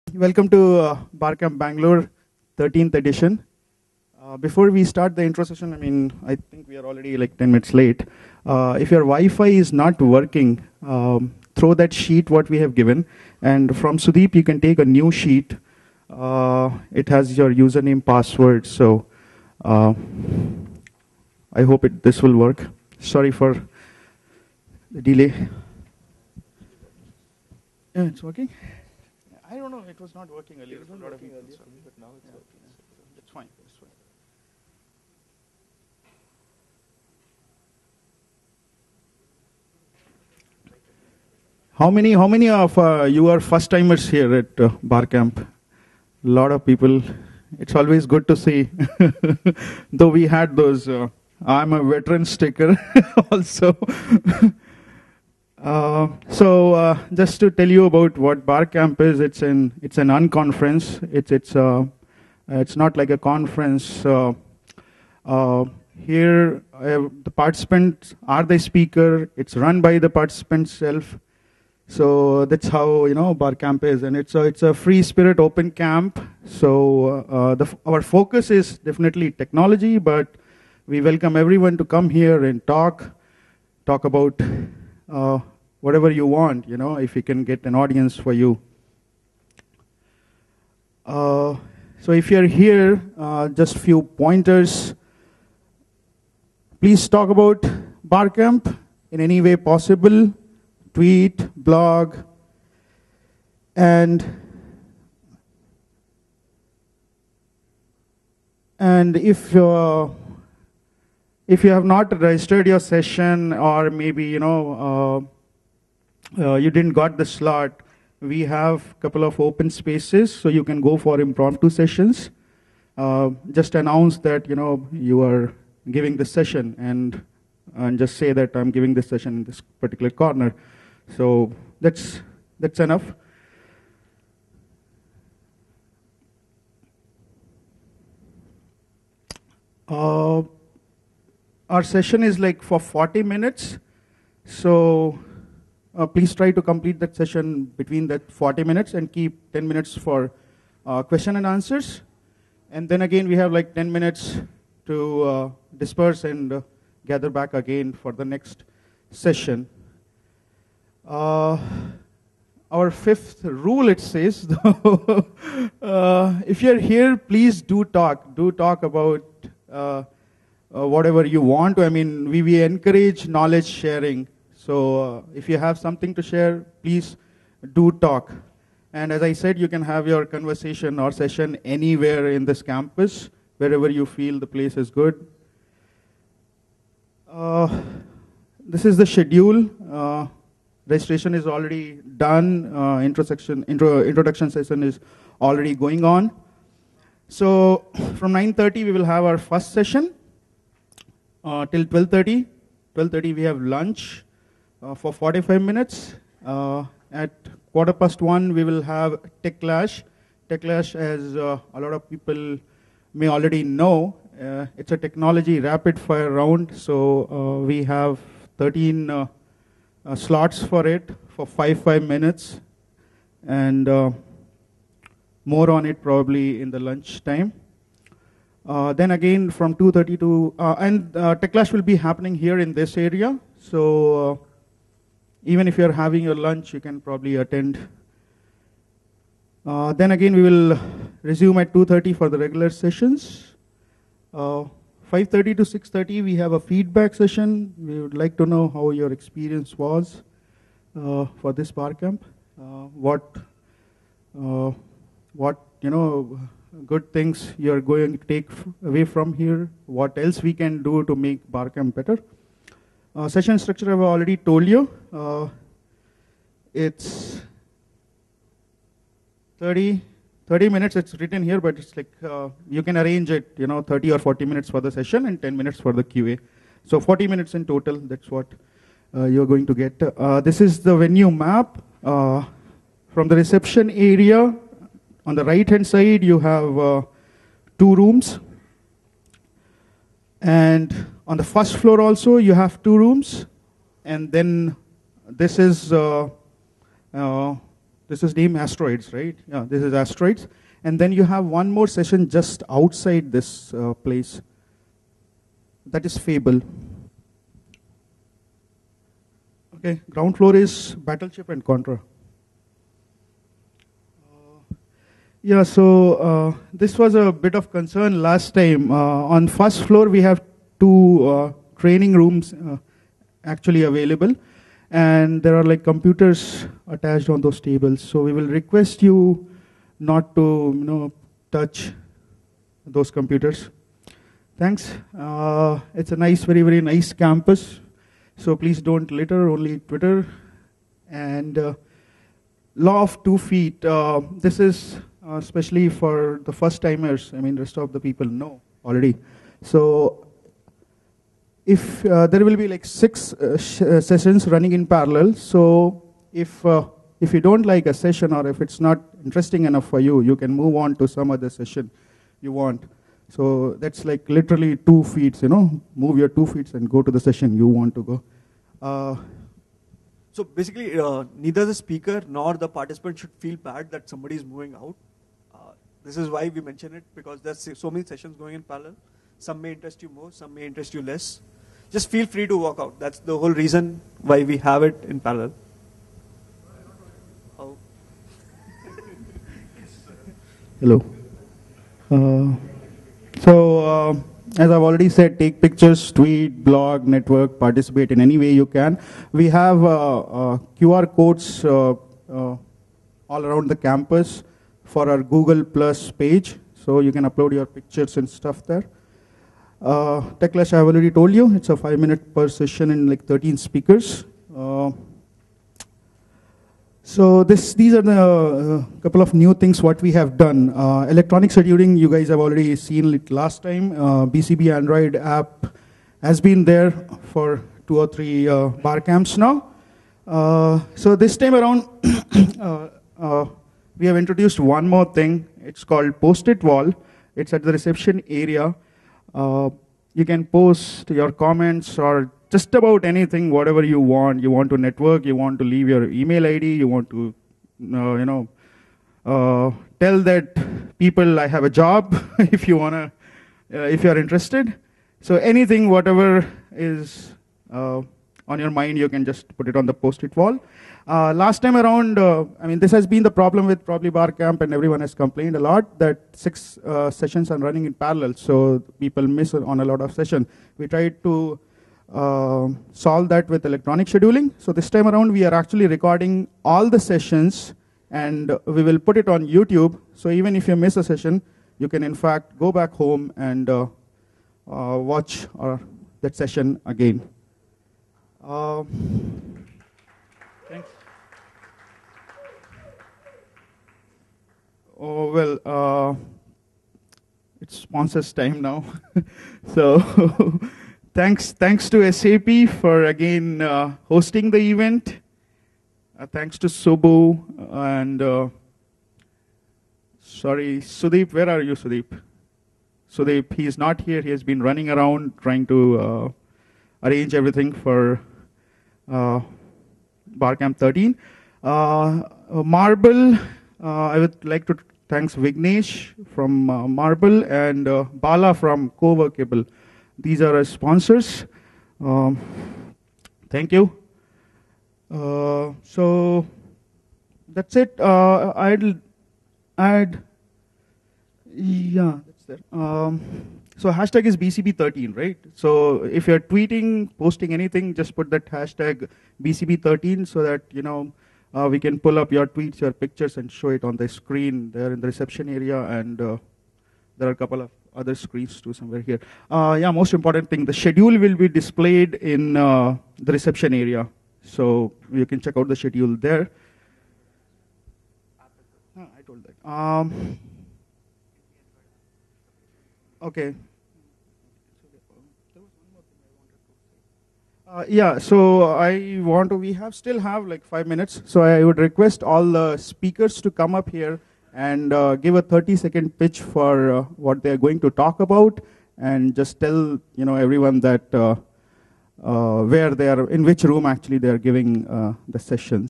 Barcamp Bangalore 13 : Intro
Amit Khare gives a brief intro about what Barcamp is and what you can expect out of it.